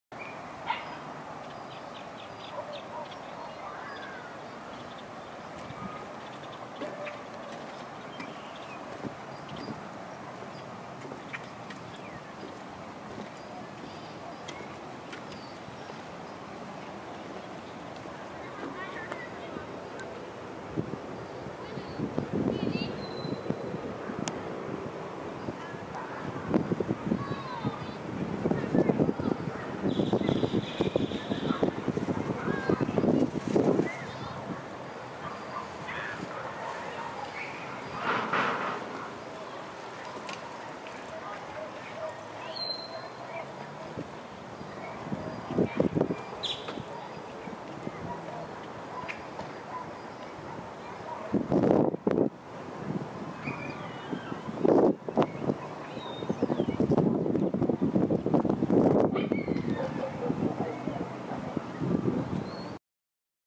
reclining_bicycle
sony tx7 hd (avchd) video test euless midway park reclining bicycle rider